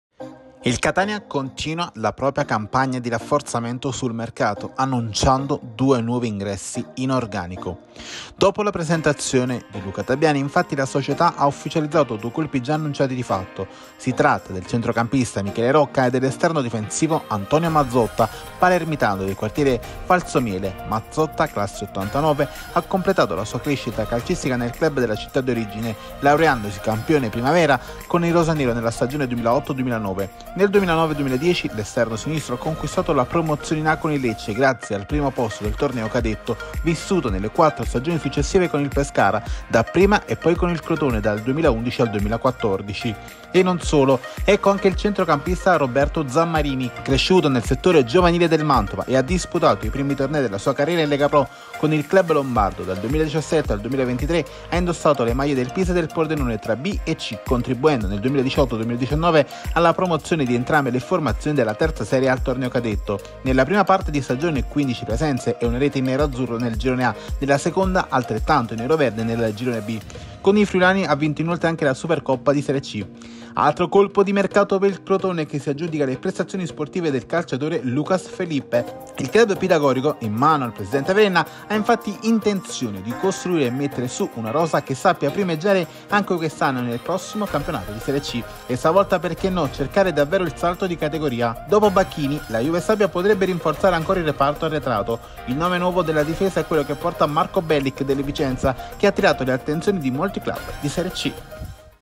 Catania scatenato in entrata, Crotone ecco Felippe
Doppio colpo in casa etnei, ecco Mazzotta e Zammarini.  Biennale per il centrocampista cercato anche dall'Avelino